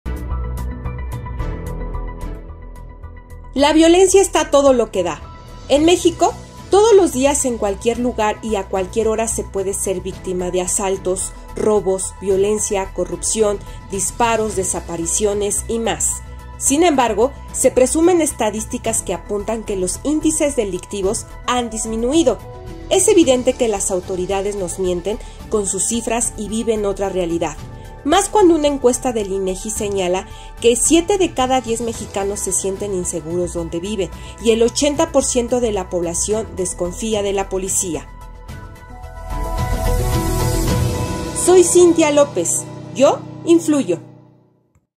La violencia está a todo lo que da
En junio de 2023, 68.6 % de las mujeres y 54.8 % de los hombres consideraron que es inseguro vivir en su ciudad.